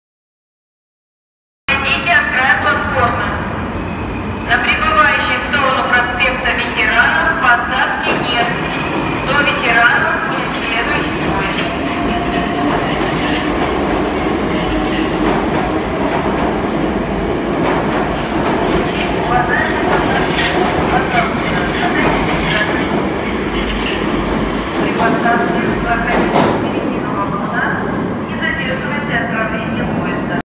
Метро-Поезд (81-722.3) (ОЭРВЗ) "Зелёный-Юбилейный"
Приписка:
ТЧ-3 "Невское"
По Маршруту
№343 (Депо) -
"Рыбацкое - Маяковская - (ССВ)- Площадь Востания -
(ССВ) - Маяковская - Рыбацкое - ТЧ-3 "Невское""
На Станции "Площадь Востания"
Перегон
Маяковская - Площадь Востания - Маяковская
(ССВ)
По 1 Пути
Линия "Кировско-Выборгская"
Санкт-Петербург,Россия.